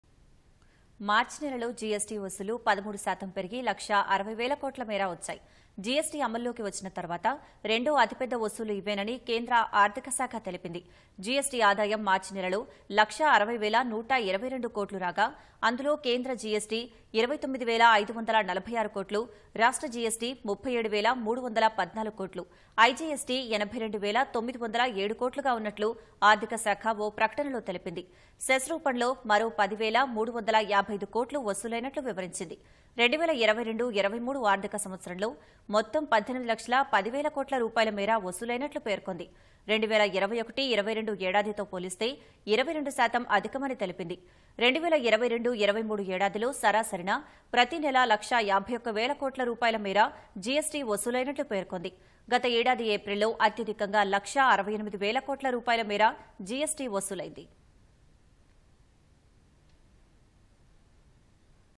India's March GST Collection Rises to Second-Highest Rs 1.60 Lakh Crore | పెరిగిన GST ఆదాయం
మార్చి నెలలో GST వసూళ్లు 13 శాతం పెరిగి. లక్షా 60 వేల కోట్లు మేర వచ్చాయి. GST అమల్లోకి వచ్చిన తర్వాత రెండో అతిపెద్ద వసూళ్లు ఇవేనని. కేంద్ర ఆర్థిక శాఖ తెలిపింది. GST ఆదాయం మార్చి నెలలో లక్షా 60 వేల 122 కోట్లు రాగా. అందులో కేంద్ర GST 29వేల 546 కోట్లు, రాష్ట్ర GST 37వేల 314 కోట్లు, I.G.S.T 82వేల 907 కోట్లుగా ఉన్నట్లు ఆర్థిక శాఖ ఓ ప్రకటనలో తెలిపింది. సెస్ రూపంలో మరో 10వేల 355 కోట్లు వసూలైనట్లు వివరించింది. 2022-23 ఆర్థిక సంవత్సరంలో మొత్తం 18 లక్షల 10 వేల కోట్ల రూపాయల మేర వసూలైనట్లు పేర్కొంది. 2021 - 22 ఆర్థిక సంవత్సరంతో పోలిస్తే 22 శాతం అధికమని తెలిపింది. 2022-23 ఏడాదిలో సరాసరిన ప్రతి నెల లక్షా 51 వేల కోట్లు రూపాయల మేర GST వసూలైనట్లు పేర్కొంది. గత ఏడాది ఏప్రిల్ లో అత్యధికంగా లక్షా 68 వేల కోట్ల రూపాయల మేర GST వసూలైంది.